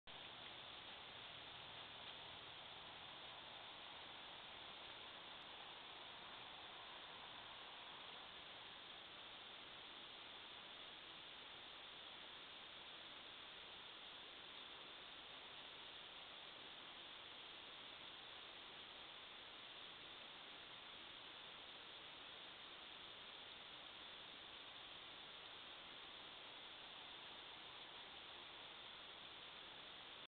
Bear Cubs
Video of two young bears twenty minutes before one is shot by fishermen in Bird Creek.